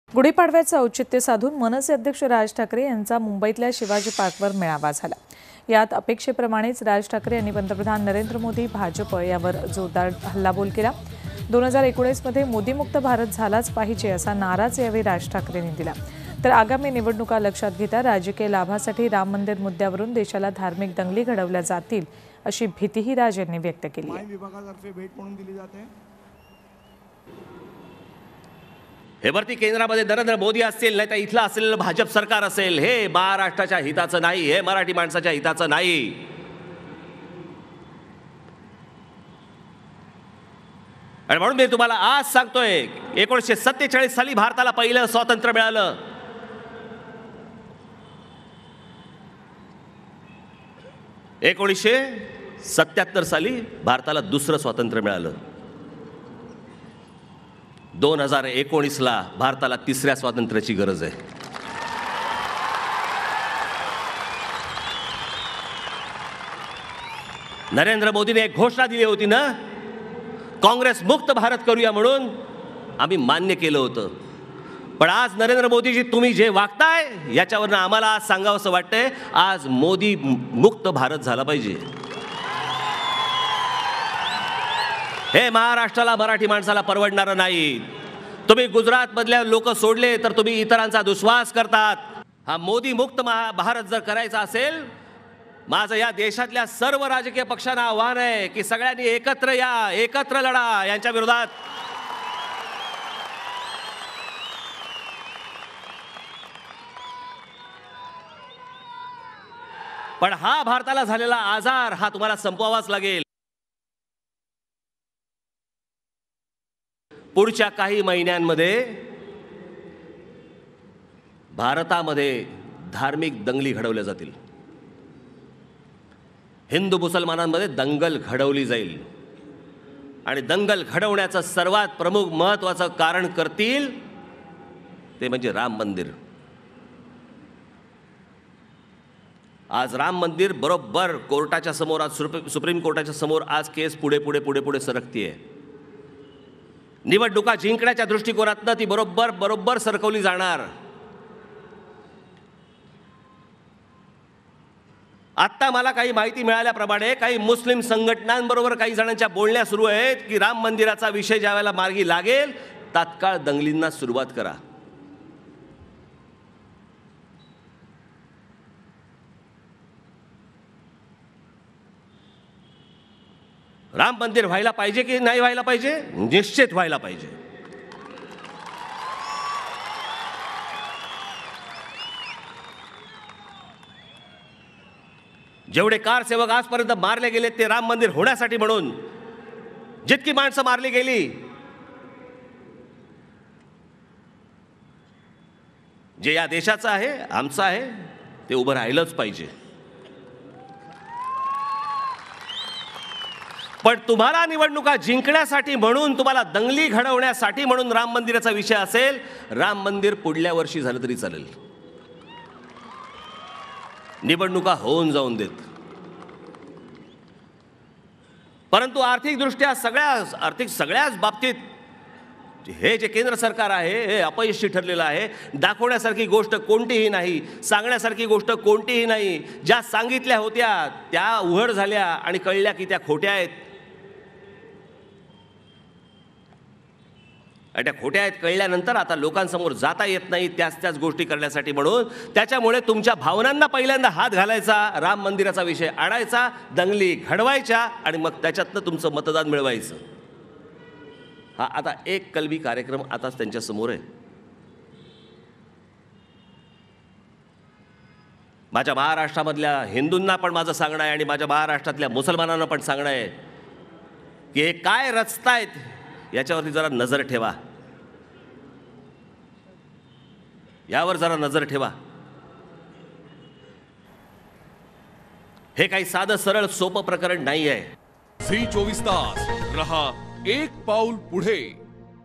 मुंबई | २०१९ मध्ये मोदीमुक्त भारताचे राज ठाकरेंनी केलं आवाहन
Mumbai,Shivaji Park Raj Thackeray Criticise To BJP Govt